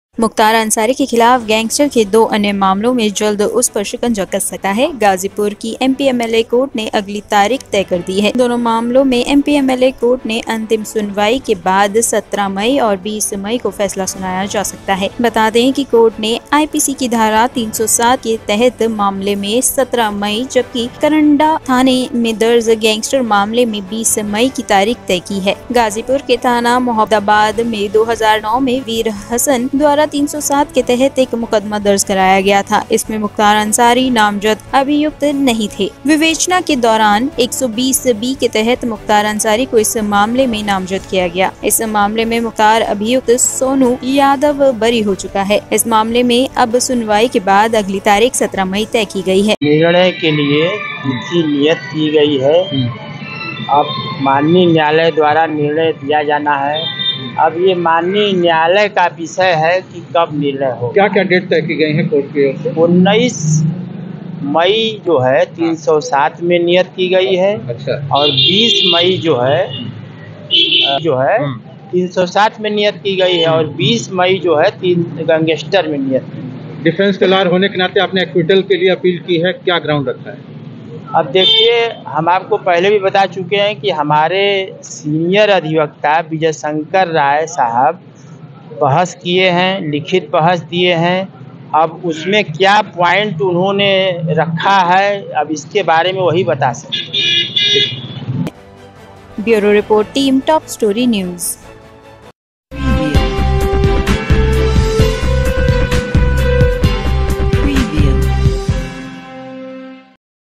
Mukhtar Ansari: मुख्तार अंसारी के खिलाफ गैंगस्टर के दो मामलों में 17 और 20 मई को आएगा फैसला | TOP
Mukhtar Ansari: मुख्तार अंसारी के खिलाफ गैंगस्टर के दो मामलों में 17 और 20 मई को आएगा फैसला, बढ़ेंगी मुश्किलें

मुख्तार अंसारी के खिलाफ गैंगस्टर के दो अन्य मामलों में जल्द उस पर शिकंजा कस सकता है. गाजीपुर की एमपी एमएलए कोर्ट ने अगली तारीख तय कर दी है. दोनों मामलों में एमपी एमएलए कोर्ट में अंतिम सुनवाई के बाद 17 मई और 20 मई को फैसला सुनाया जा सकता है